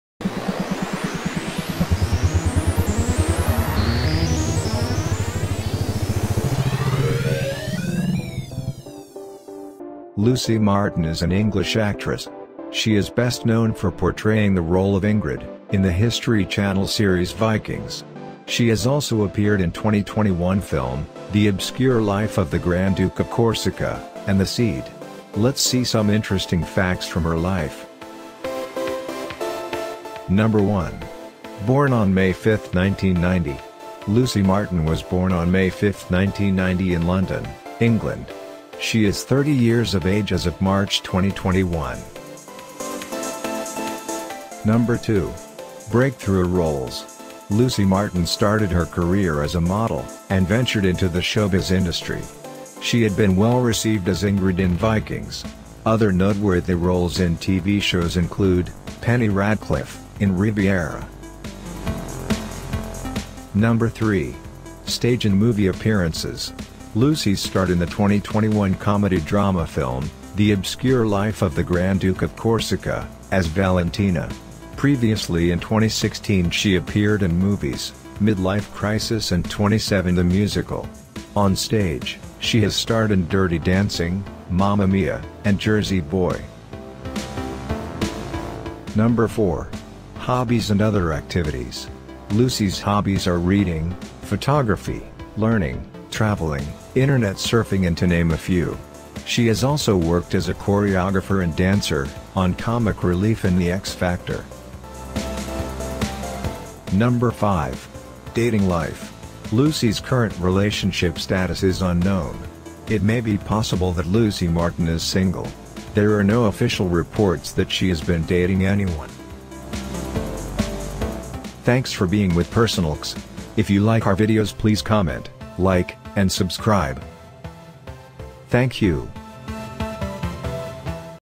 Five things you may not know about Lucy Martin.
In this PersonalX video we are answering the following questions and something more about Lucy Martin:

When and where was Lucy Martin born?
How did Lucy Martin start acting?
What are Lucy Martin 's breakthrough roles?
What are Lucy Martin 's accomplishments in television, on stage and movies ?
Who is Lucy Martin 's boyfriend?
What are Lucy Martin 's hobbies?